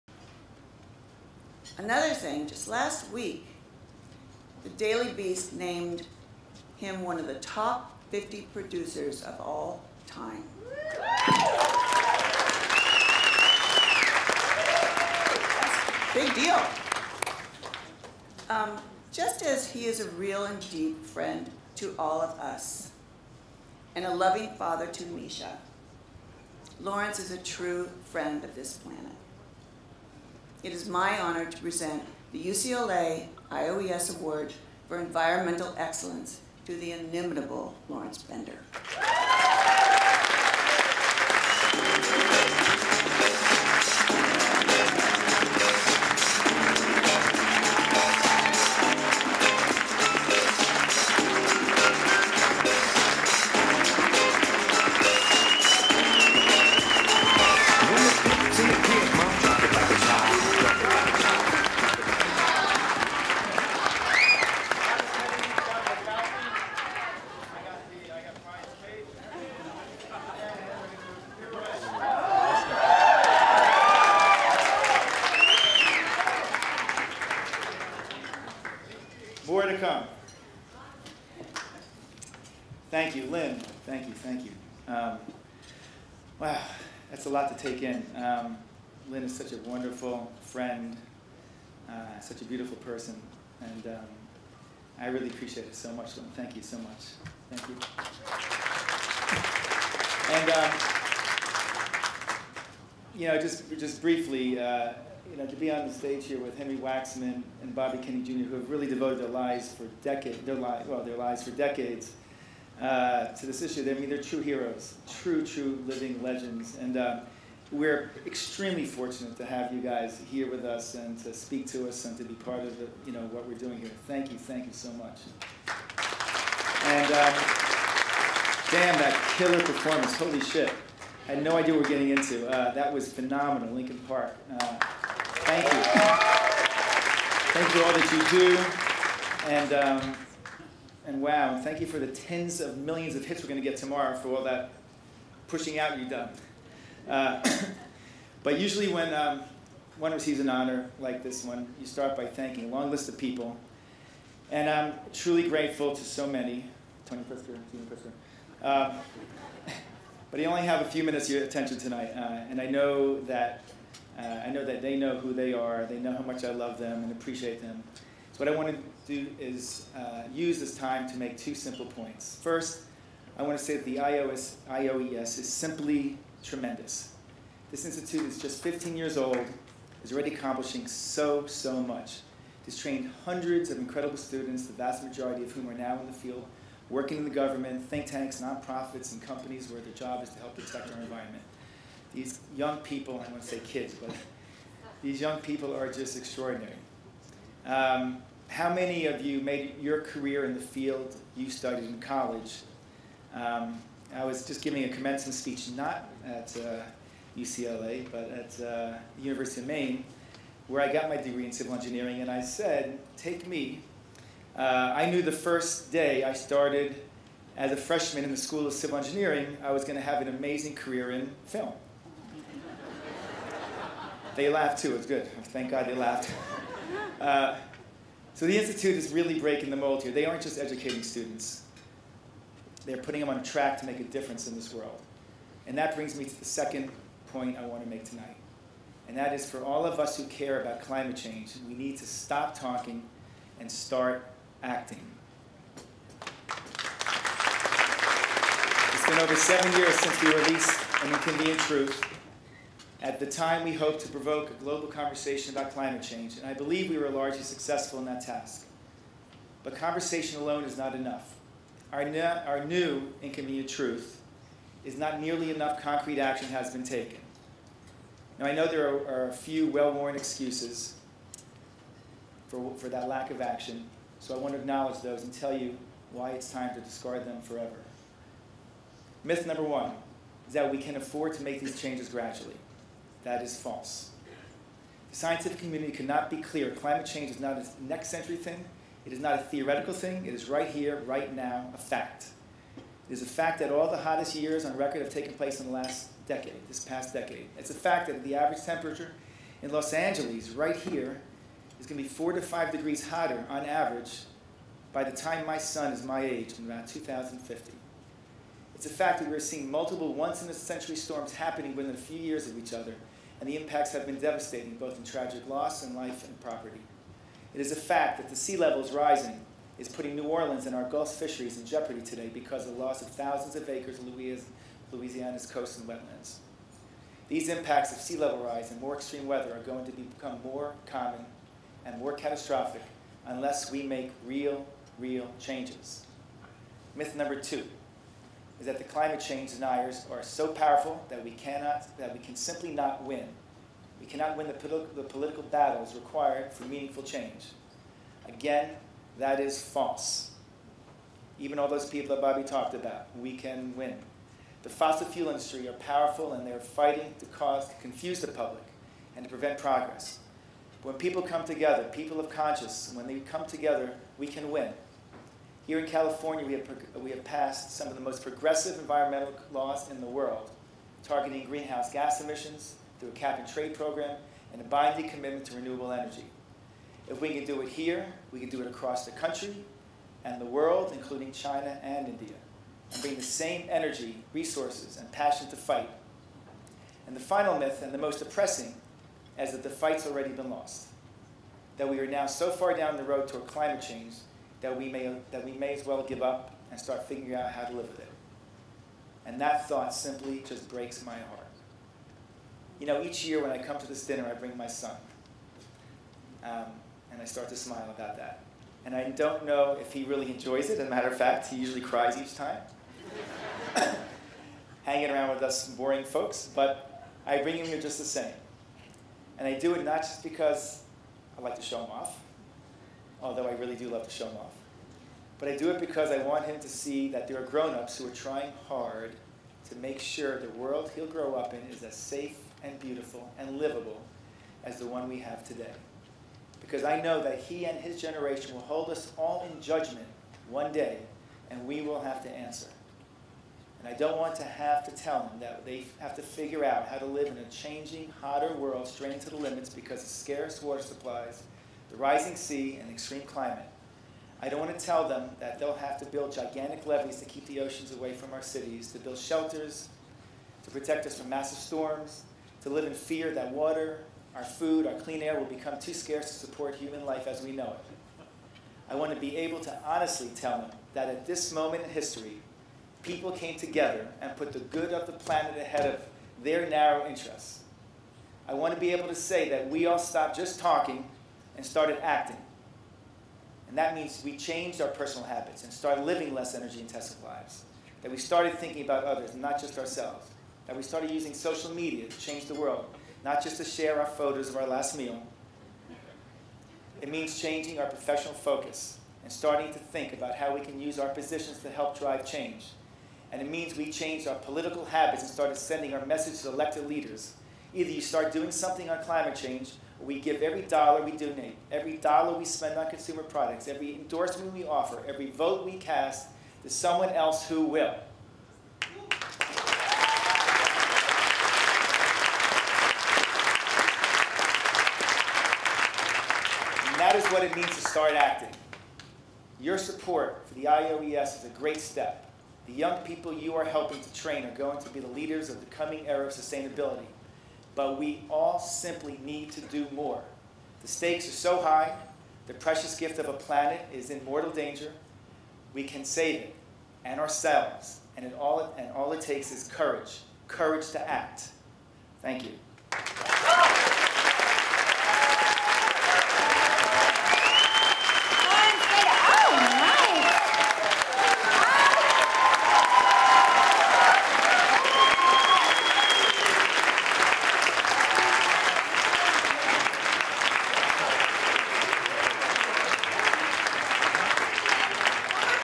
Evening of Environmental Excellence Speech - Lawrence Bender
Activist and philanthropist Lyn Lear presented producer Lawrence Bender with the Emerald Award for Environmental Leadership at the Institute's third annual gala held March 21, 2014. Video by Erick Weiss.